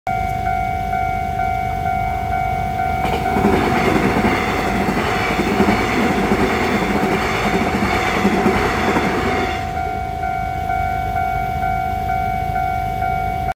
台鐵2154次EMU700型區間車經山線往基隆永豐路平交道通過
拍攝器材：Samsung Galaxy A55